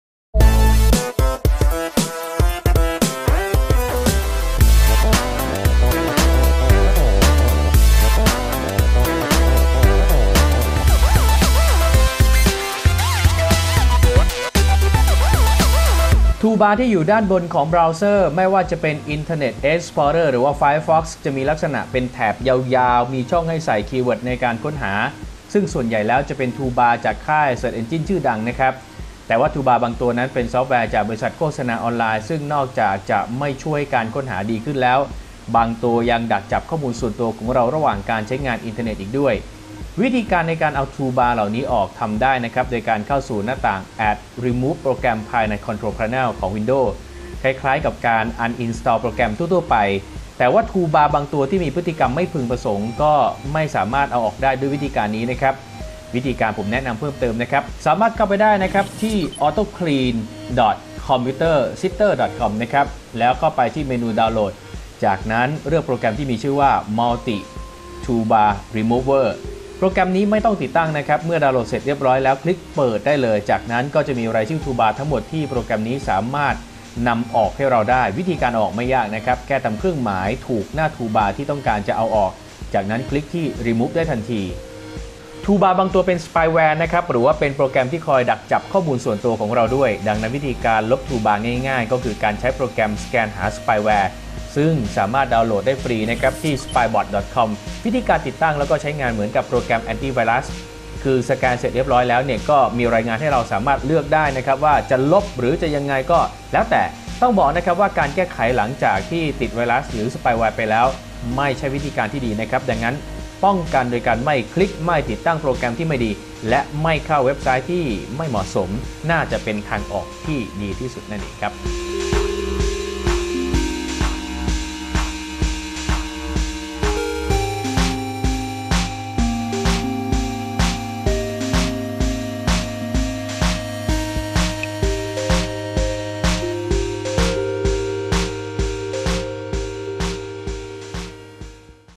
ทูลบาร์เยอะ เอาออกยังไง
สำหรับคนที่ใช้งานอินเทอร์เน็ตบ่อย ๆ บางครั้งก็มีการติดตั้งซอฟแวร์ใหม่ๆ .. บ่อยครั้งมีทูลบาร์ที่ไม่ได้ตั้งใจ ติดตั้งเข้ามาในบราวเซอร์ด้วย นับวันก็มีทูลบาร์หลายเจ้าเต็มไปหมด แต่ก็ไม่เคยได้ใช้งาน